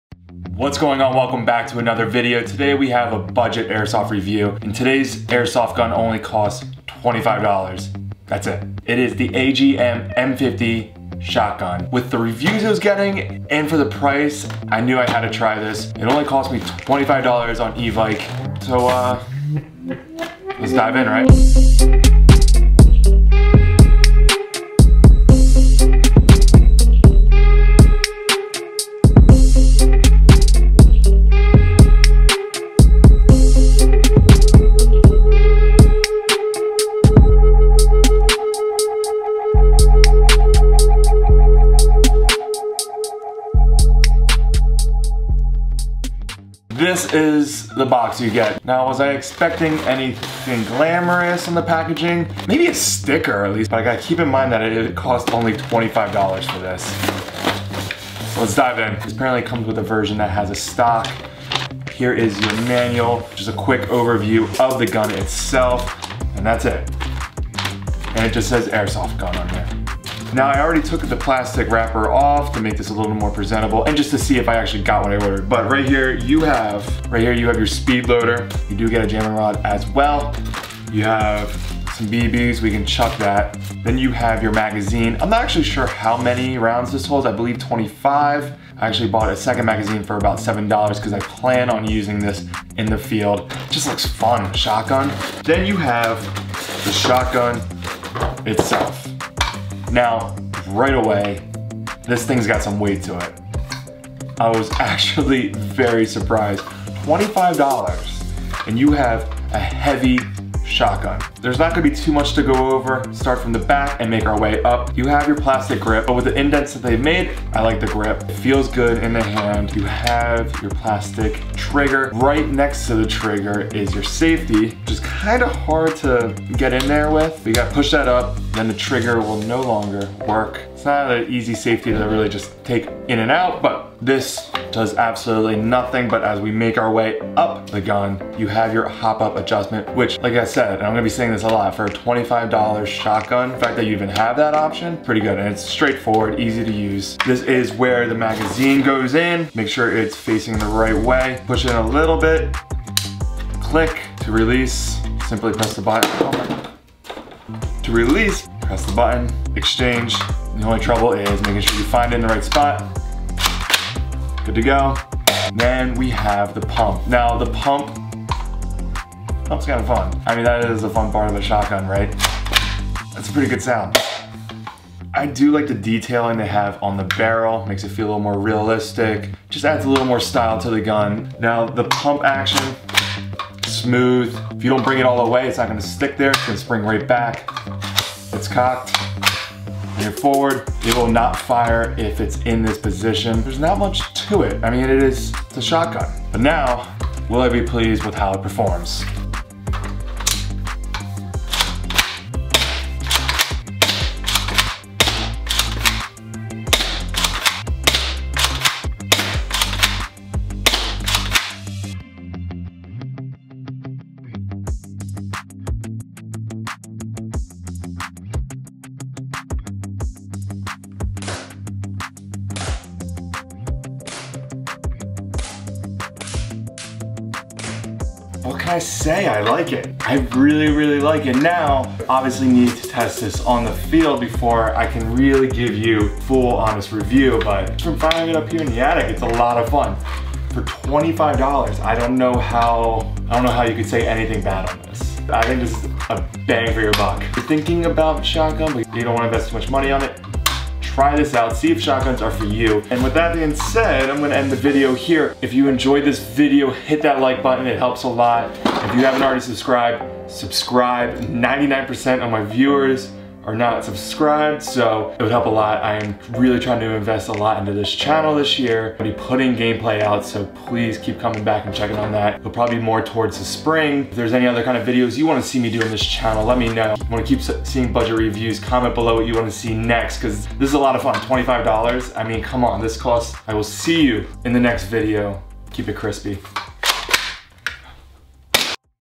$25 AGM M-500 Airsoft Shotgun!!!
In today's budget review I unbox the AGM M-500 airsoft shotgun. This can be bought for only $25!!!

Features:
- Skirmish grade Airsoft with great power and accuracy
- Metal heat sink and reinforced internal parts
- On-frame Precise Hop-up Adjustment Switch
- Realistic heavy weight
- Dramatic sure-cocking forearm with ergonomic grip and realistic pump action
- Detachable magazine
- Rugged and Robust Construction

SOCIAL

AIRSOFT GEAR

CM Raider
Walther PPQ
Second Goggles
Second Gloves
Red Dot Optic

CAMERA GEAR